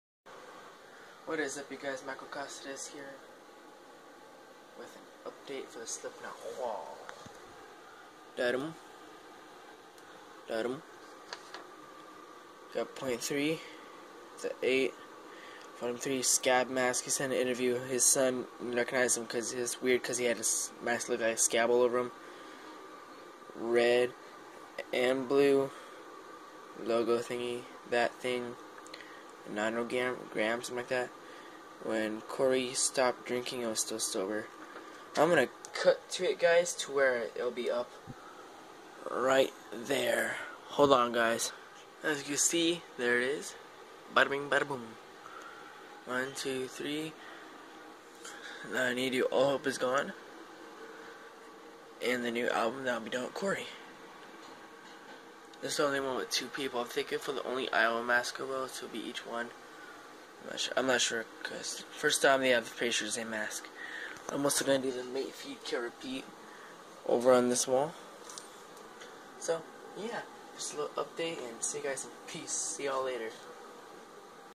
SLIPKNOT WALL!!! COREY IS ALMOST DONE!!!
Updating you on the SLIPKNOT wall and the current status of it all. PEACE YALL!!!